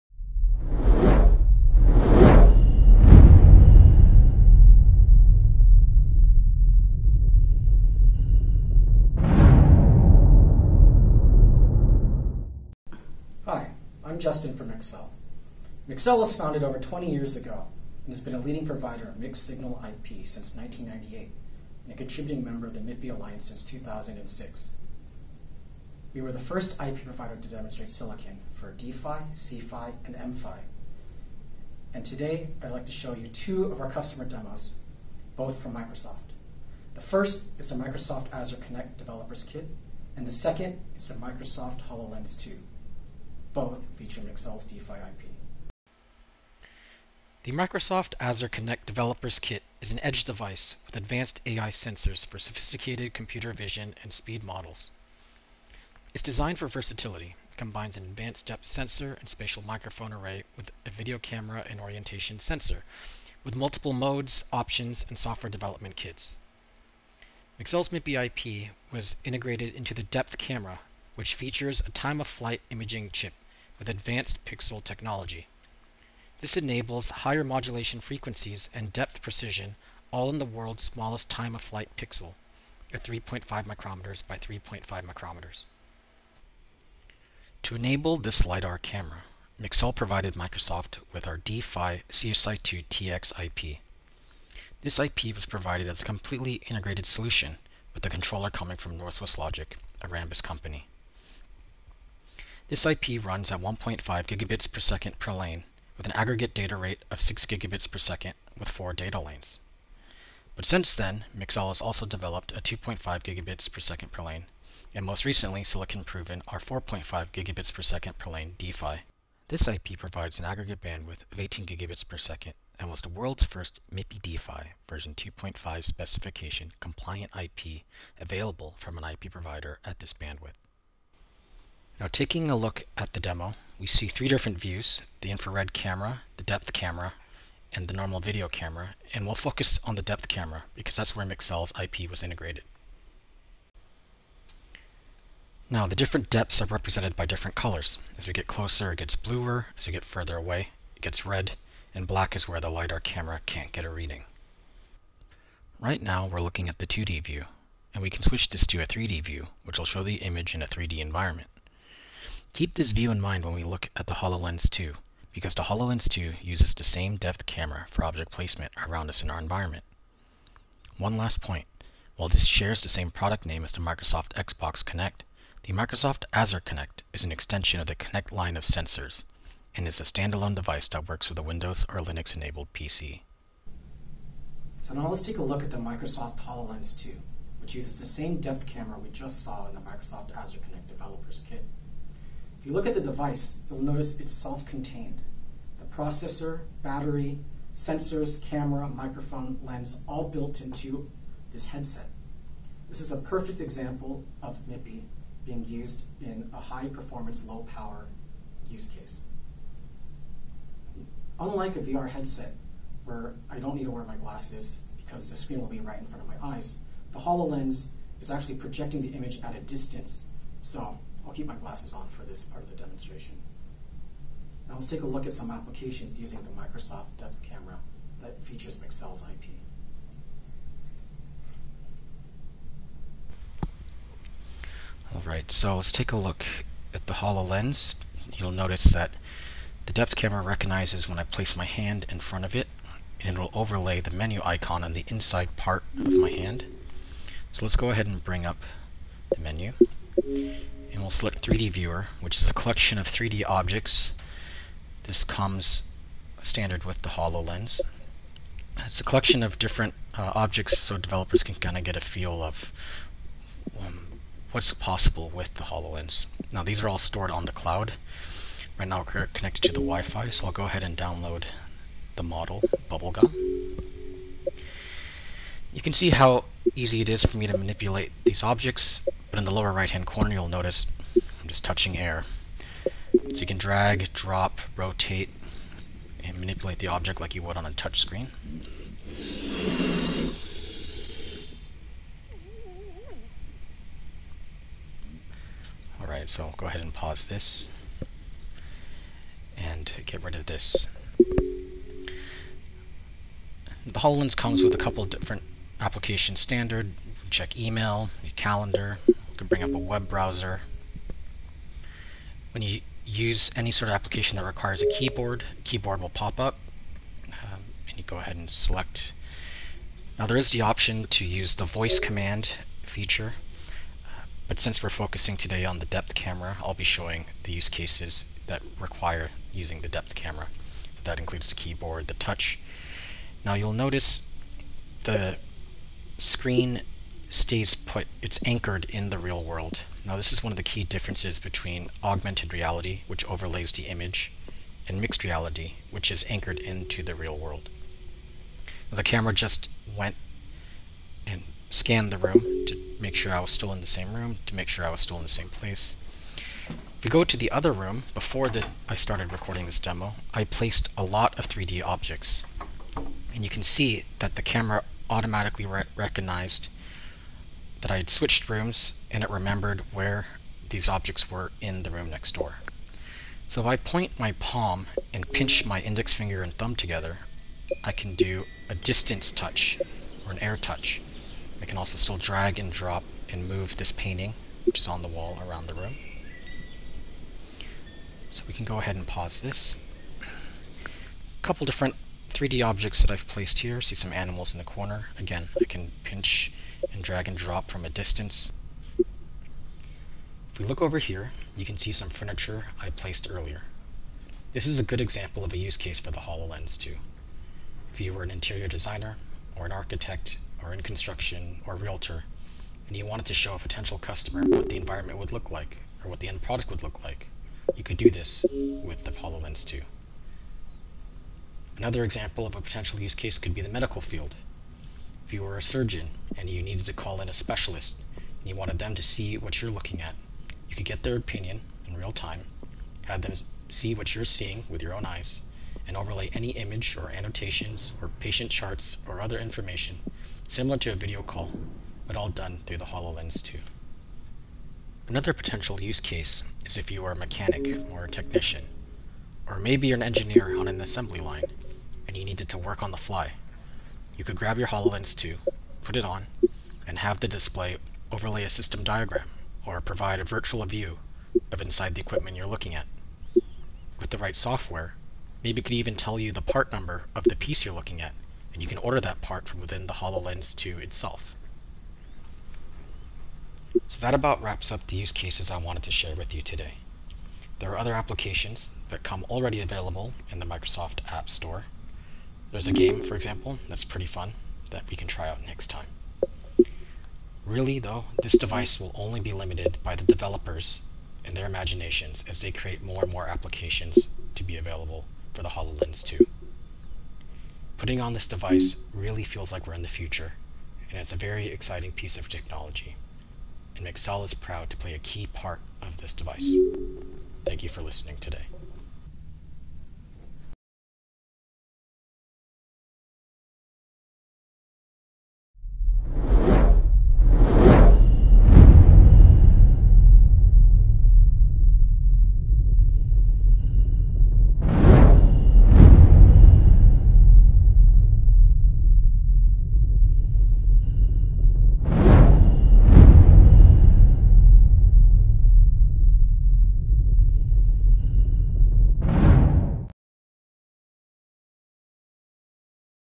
Microsoft HoloLens 2 Demo featuring Mixel's MIPI D-PHY IP
Mixel’s MIPI D-PHY IP integrated into Microsoft’s Azure Kinect DK and HoloLens 2 depth camera, featuring the world’s smallest Time-of-Flight pixel.

Azure Kinect Developer Kit is an edge device with advanced AI sensors for sophisticated computer vision and speech models. Designed for versatility, it combines an advanced depth sensor and spatial microphone array with a video camera and orientation sensor — with multiple modes, options, and software development kits (SDKs). The Azure Kinect DK is aimed at broad-based enterprise users and enables the developer to create interactive Virtual, Augmented, and Mixed Reality experiences.

Mixel’s MIPI IP was integrated into the 1MP depth camera, featuring a Time-of-Flight (ToF) imaging chip with advanced pixel technology. This enables higher modulation frequencies and depth precision all in the world’s smallest ToF pixel, at 3.5μm by 3.5μm. The sensor measures the depth and displays it in different colors (blue is closer, red is further, and black is out of range).

This same depth camera incorporating the Mixel IP is also a key part of the HoloLens 2.

Mixel provided the Mixel MIPI D-PHY CSI-2 TX IP. The D-PHY link can operate with 1 to 4 lanes and supports an aggregated data rate of 6 Gbps. It uses a clock-forwarded synchronous link that provides high noise immunity and high jitter tolerance. The Mixel IP incorporates proprietary differentiating features to reduce stand-by current and wake-up time.